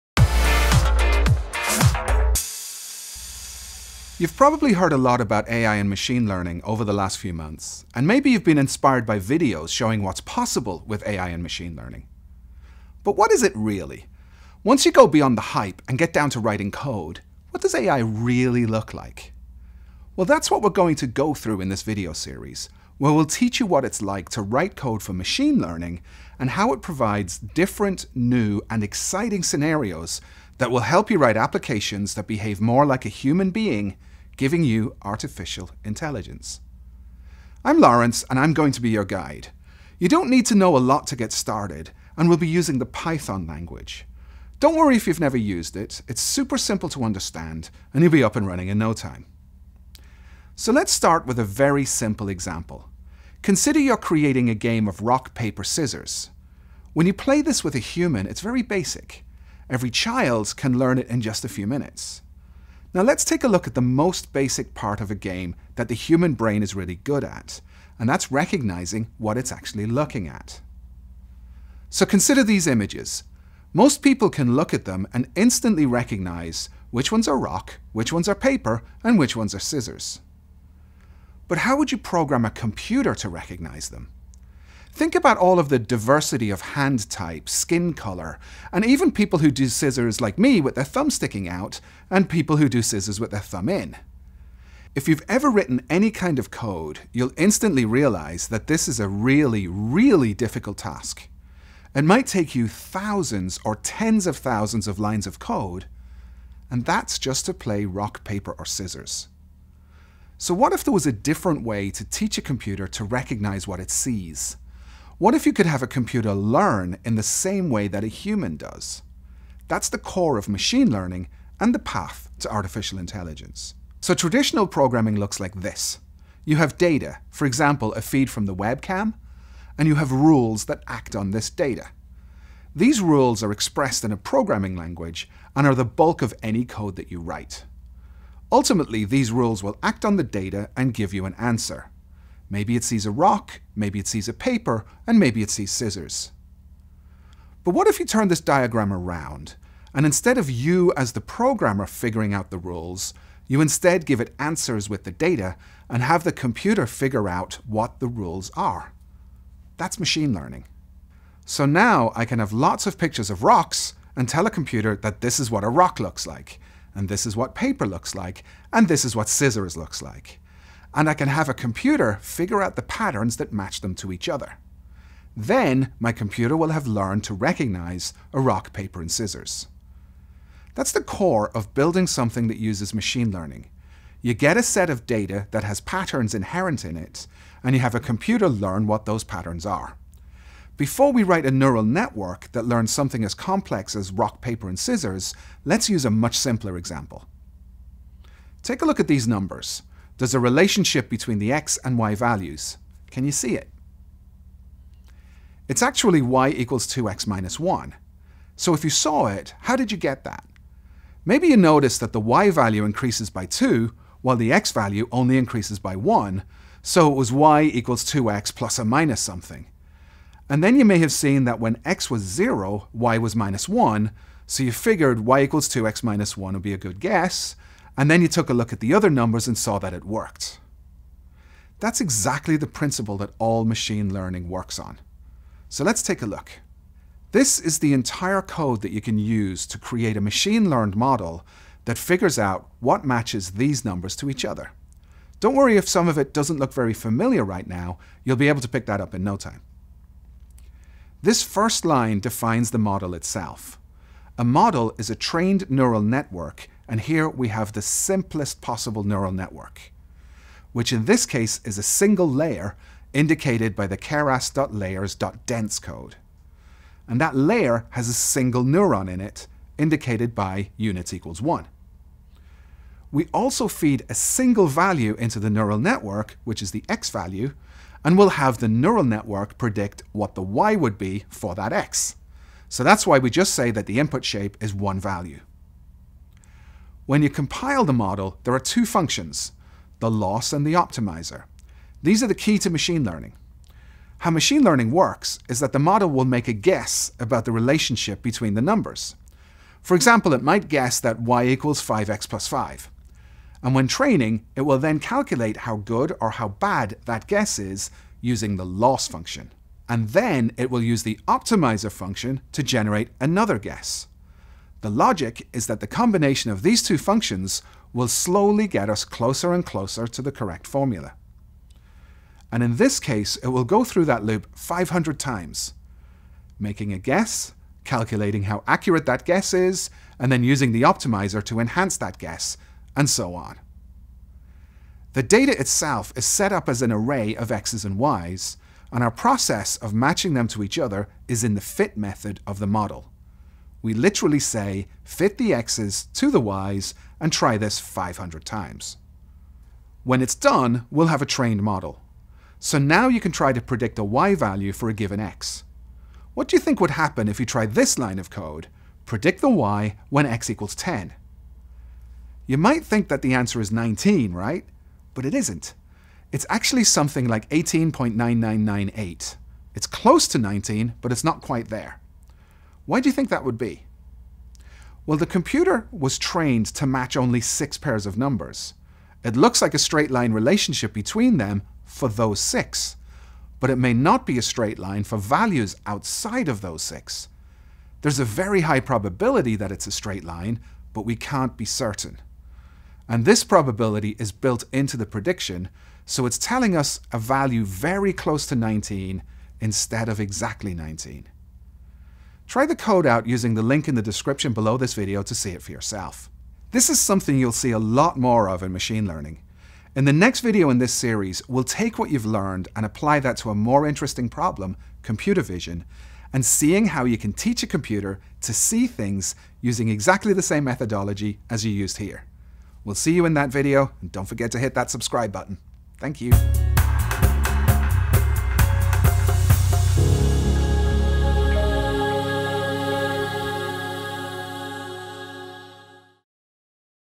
Intro to Machine Learning (ML Zero to Hero - Part 1)
Machine Learning represents a new paradigm in programming, where instead of programming explicit rules in a language such as Java or C++, you build a system which is trained on data to infer the rules itself. But what does ML actually look like? In part one of Machine Learning Zero to Hero, AI Advocate Laurence Moroney (lmoroney@) walks through a basic Hello World example of building an ML model, introducing ideas which we'll apply in later episodes to a more interesting problem: computer vision.

Try this code out for yourself in the Hello World of Machine Learning

This video is also subtitled in Chinese, Indonesian, Italian, Japanese, Korean, Portuguese, and Spanish.